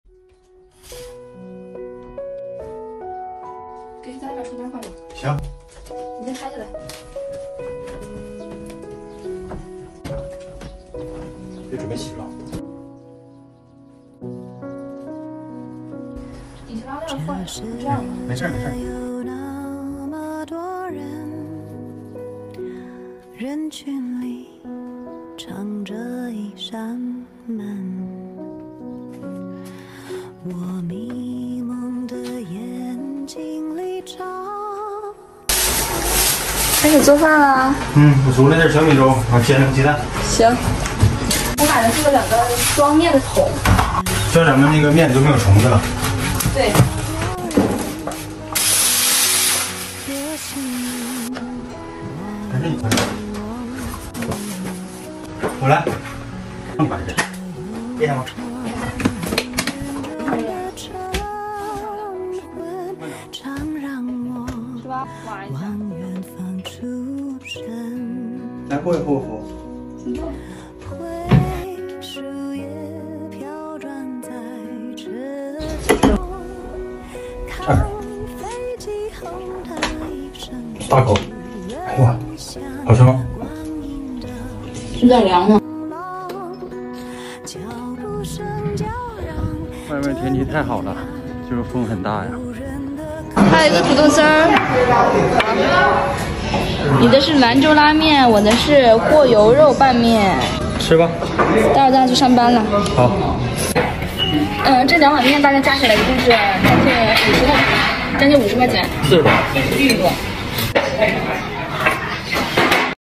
北漂情侣在北京 合租12平米卧室的生活 ，租的房子虽小，但是生活是甜的，今天和聪聪一起休息了
90后情侣的北漂vlog ！
希望我们能从无到有
四季交替 我们还是我们

The Northern Drifting vlog of 90s Couples!
I hope we can grow from scratch
Four seasons alternate, we are still us